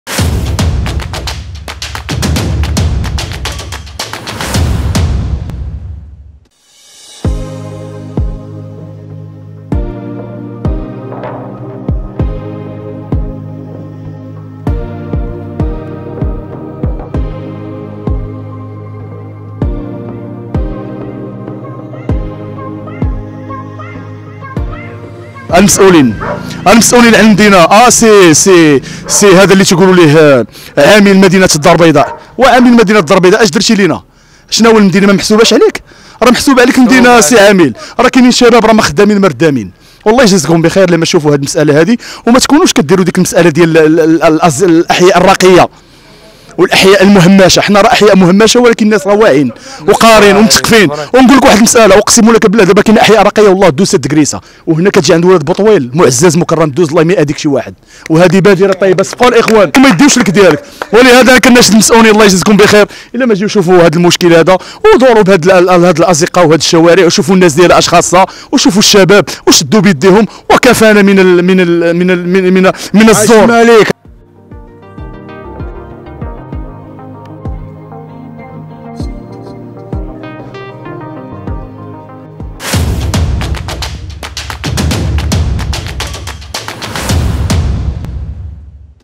والمسؤولين..شوفو من حال أولاد درب بوطويل بالبيضاء
أولاد درب بوطويل بالبيضاء" يتحدثون بحرقة حول معناتهم بسبب اهمال المسؤولين"